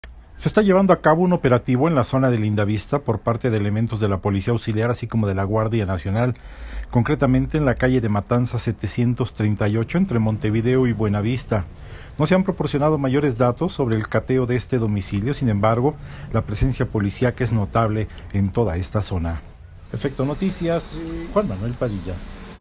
Operativo en Lindavista, CDMX
Este miércoles 18 de marzo se lleva a cabo un operativo en la zona de Lindavista por parte de la Guardia Nacional y la Policía Auxiliar